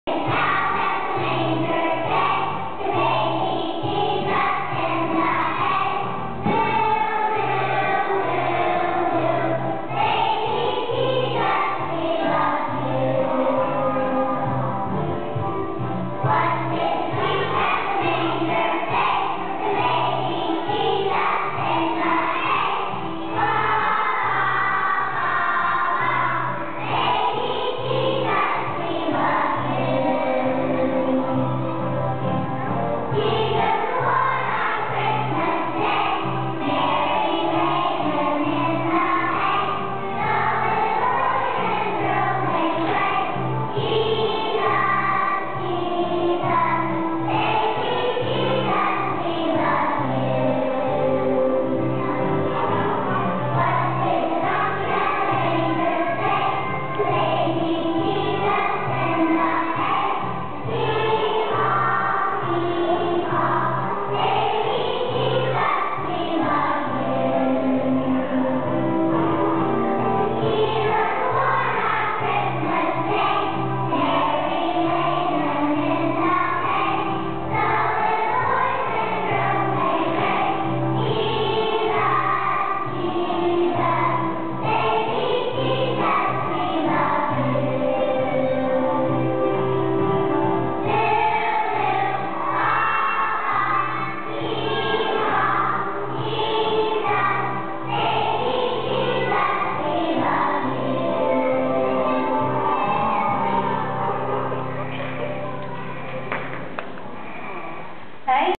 Nadia Xmas 2007
Nadia singing at Church for her school play